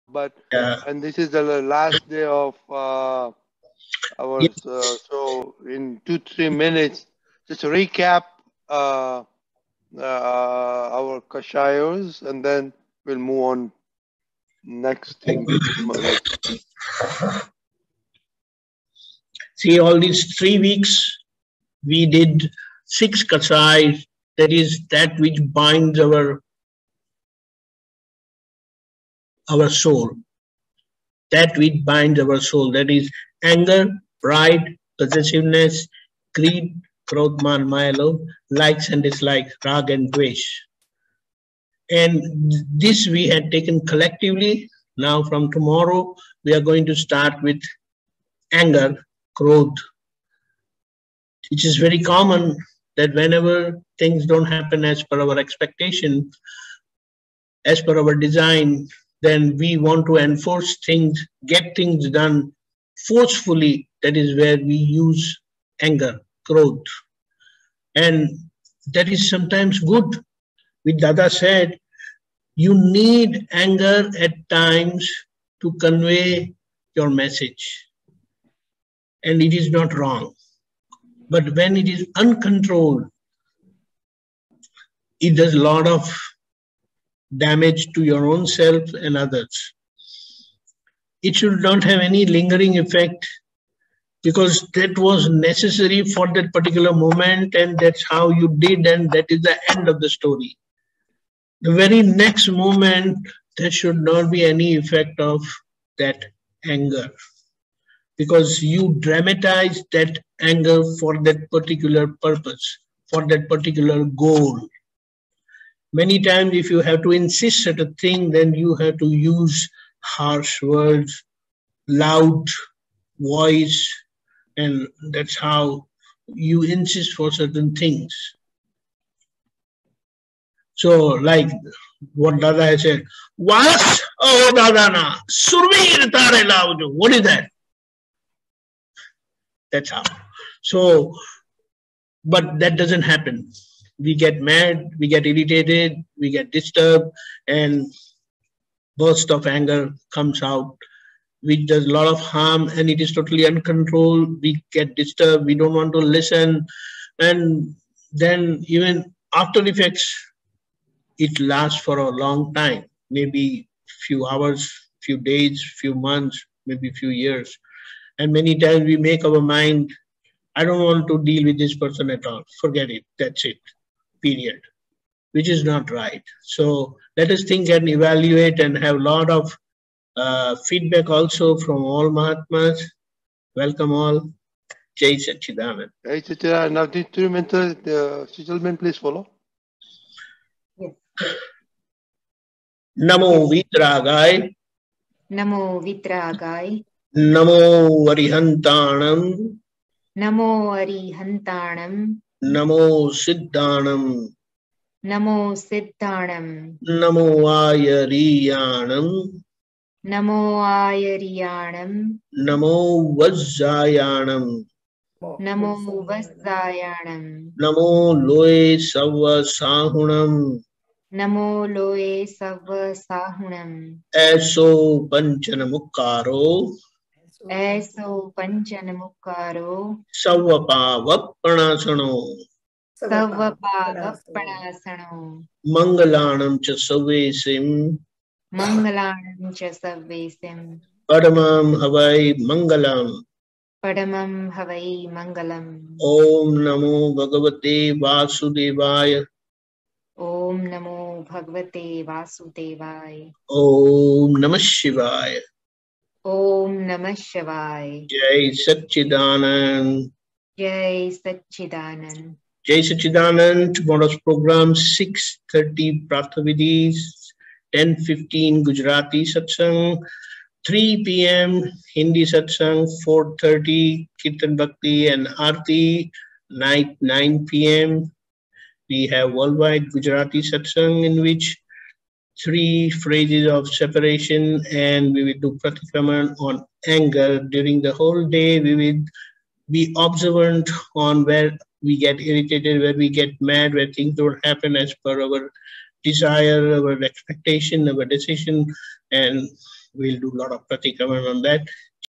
24 July 2022 Dada Bhagwan JSS Apt Depakanandji, on Samayik & Pratikraman of our Kashays
Dada Bhagwan JSS Apt Depakanandji, on Samayik & Pratikraman of our Kashays